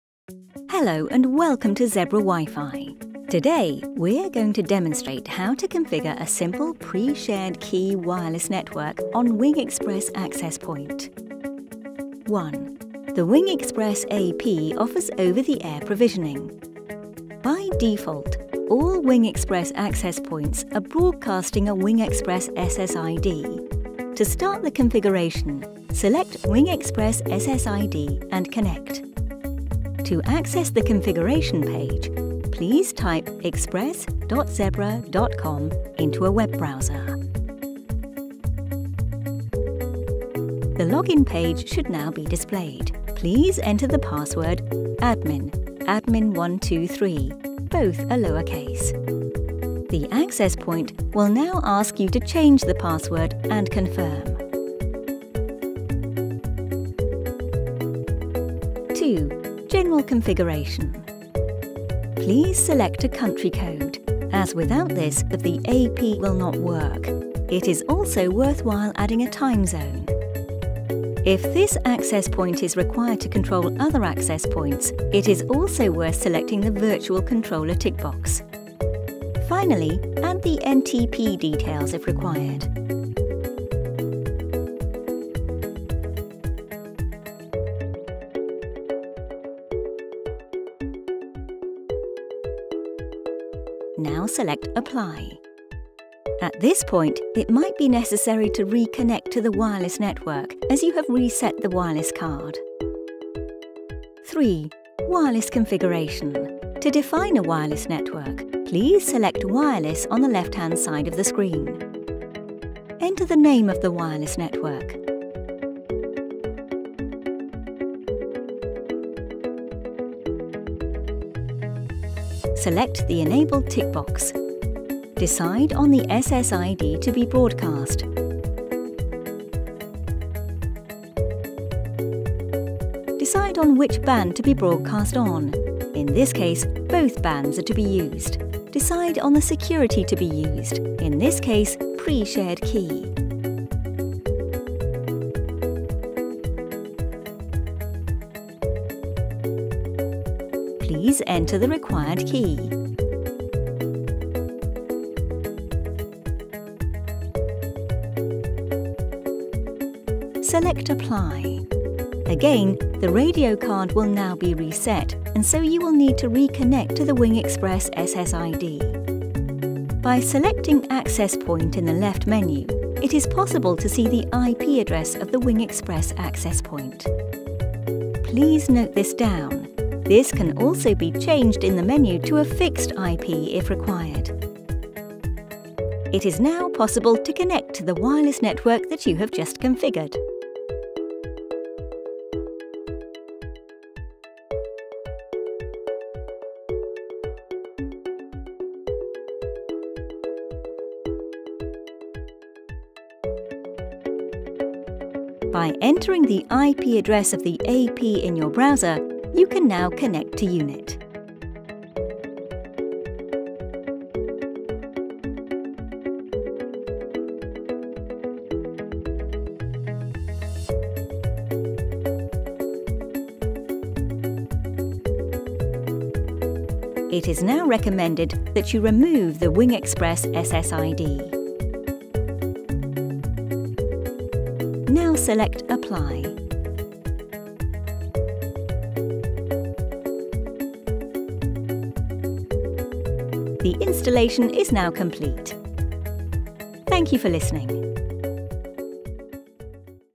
Zebra WiNG EXPRESS - Quick Install Video
ZEBRA WING EXPRESS steht für eine unkomplizierte Inbetriebnahme eines professionellen WLAN Netzwerks. Sehen Sie hier, wie einfach die Konfiguration von WING EXPRESS ist.